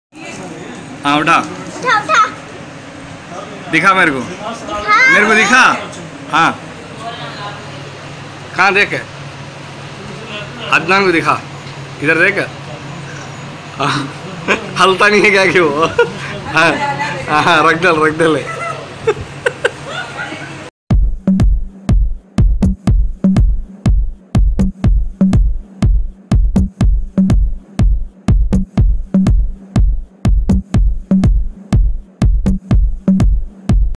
Kid Playing with Pet Tortoise
Small Turtle Swimming In Fish Bowl Baby girl Introducing Her Small tortoise pet